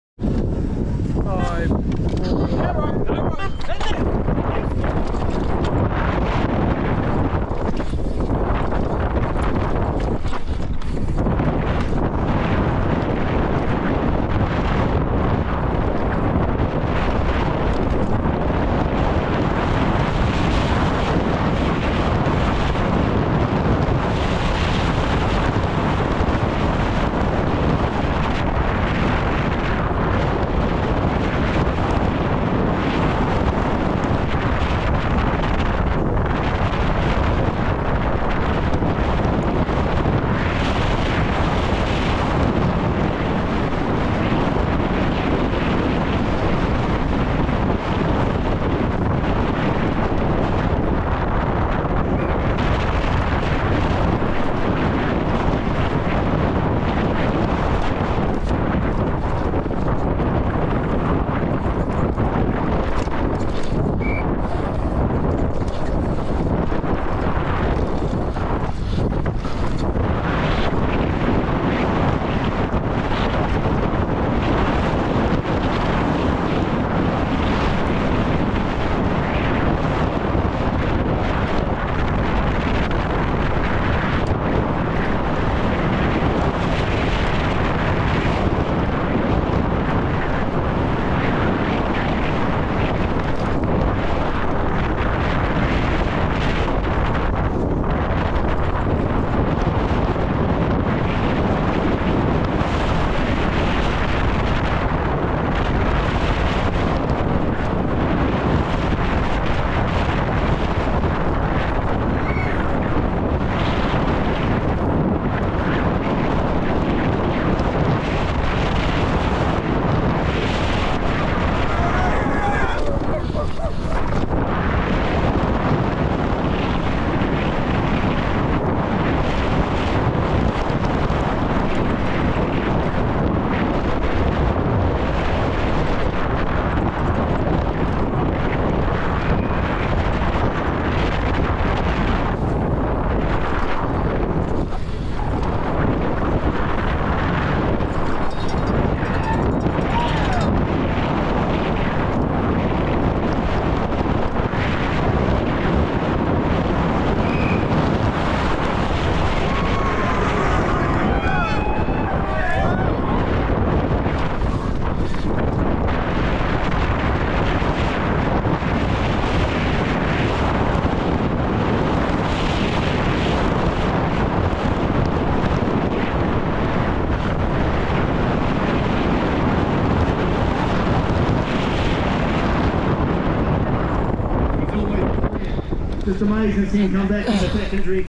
CORONET PEAK NAT CHAMPS POV FINALS RUN
Insane day racing National Champs not a great day for me but moving forward!!